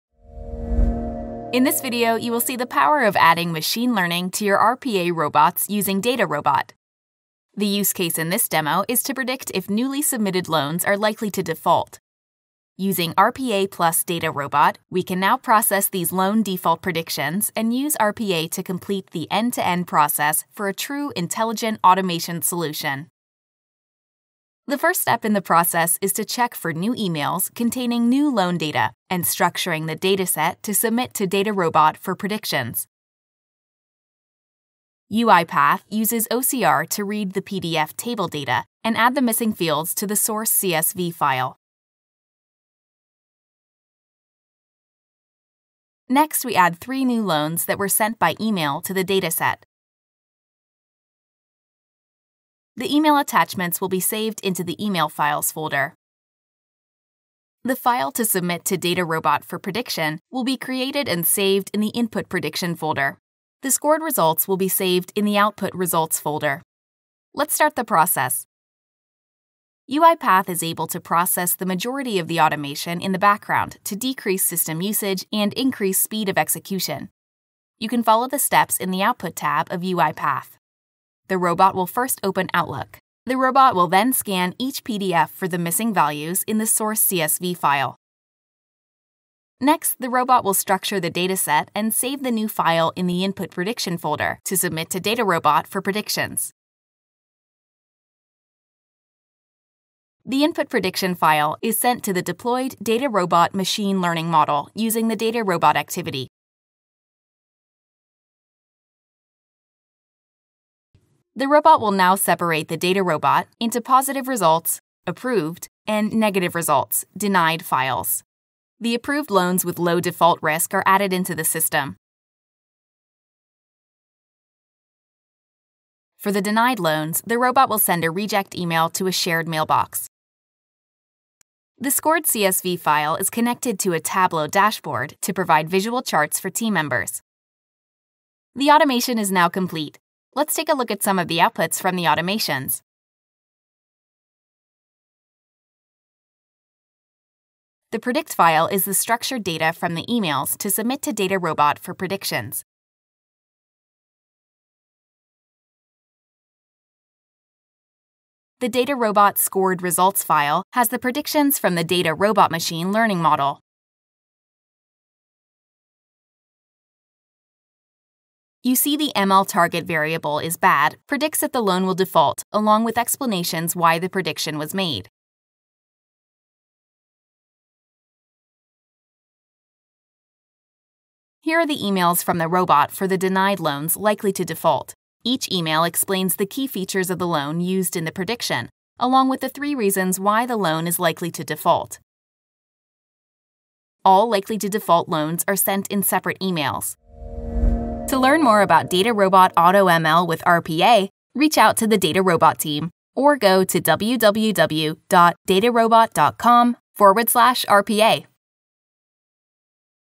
DataRobot & UiPath Demo
This video demonstrates the joint power of RPA and Automated Machine Learning.